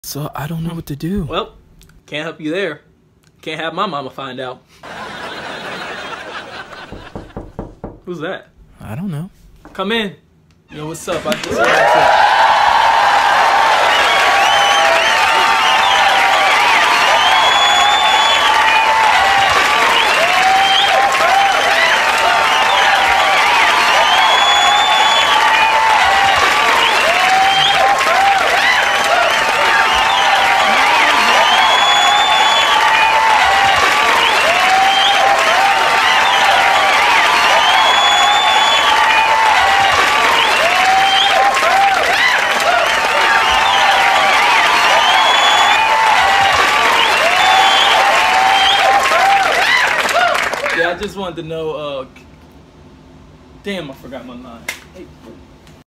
When a Celebrity makes an appearance on a sitcom show (Original Tik Tok)
This is how it really be 😂😂😂😂